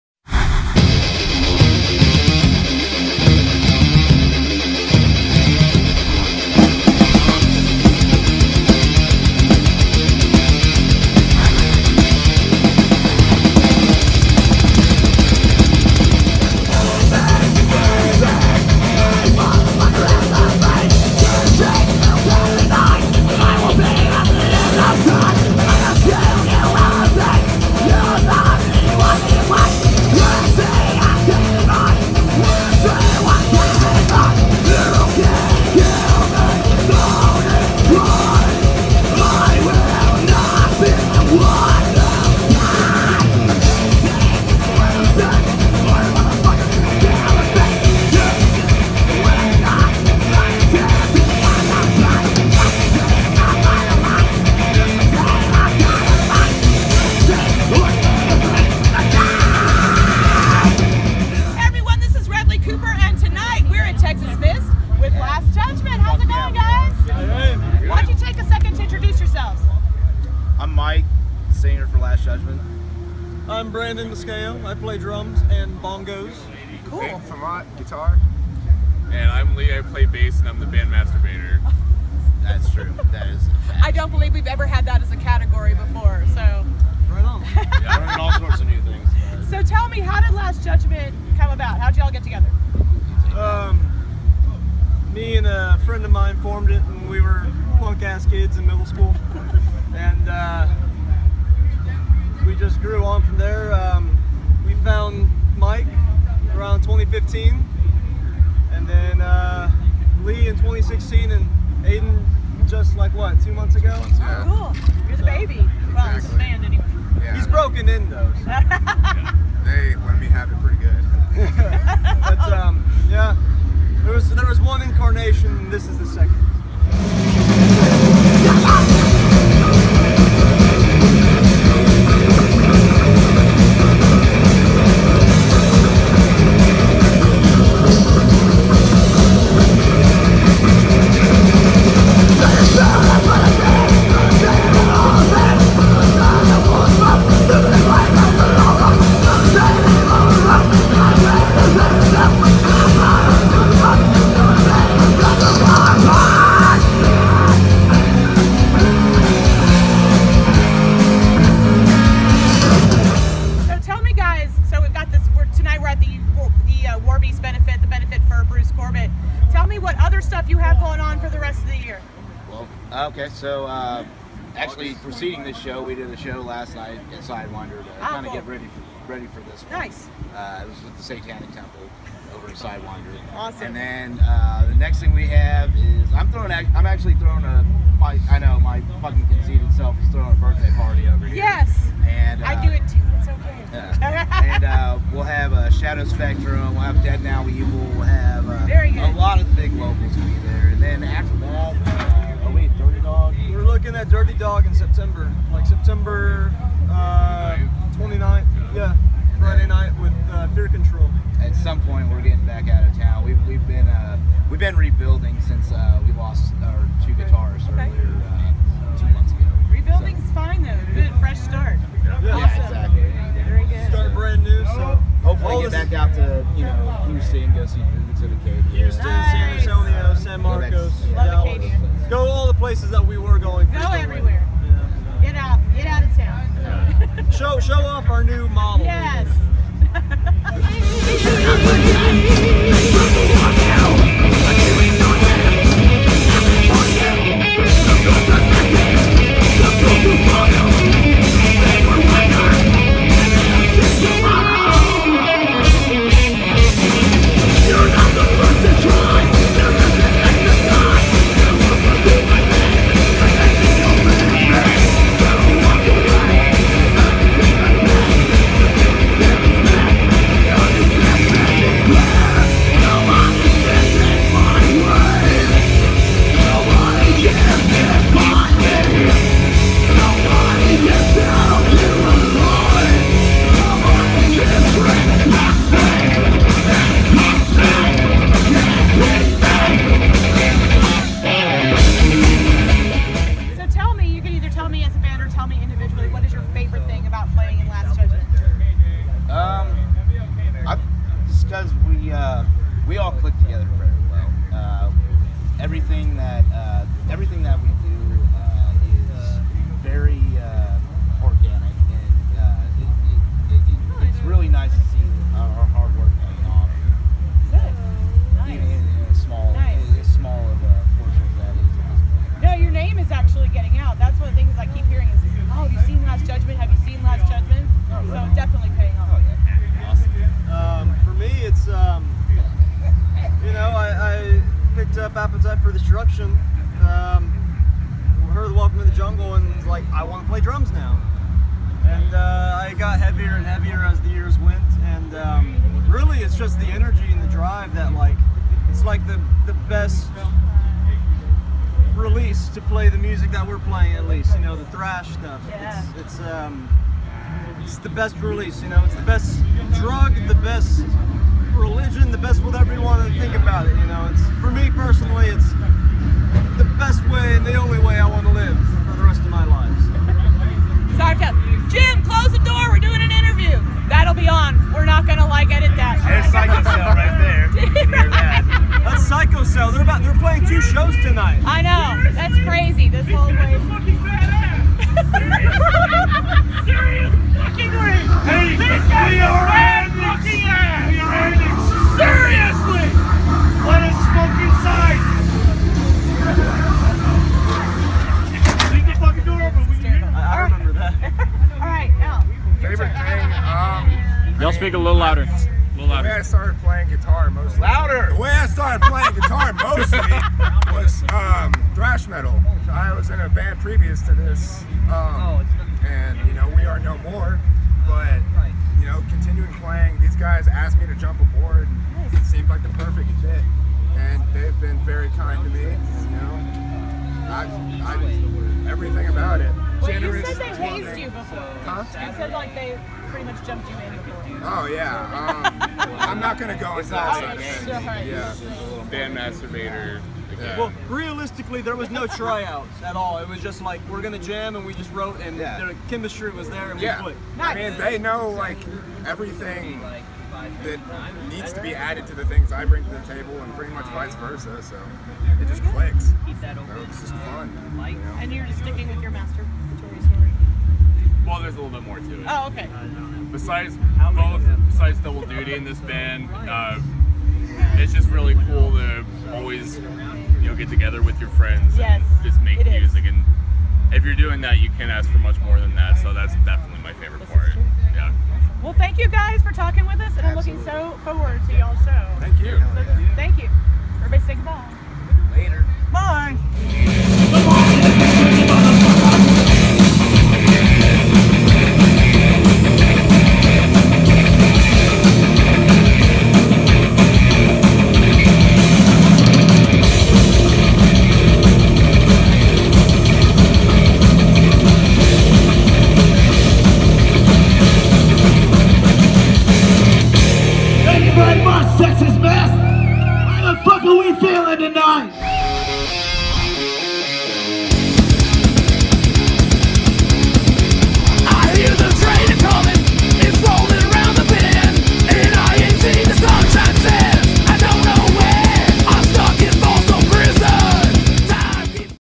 ESM's Out on the Town Ep. 43 - Last Judgement
ESM's Out on the Town Episode 43 featuring Last Judgement from their show at Texas Mist, 7/22/17.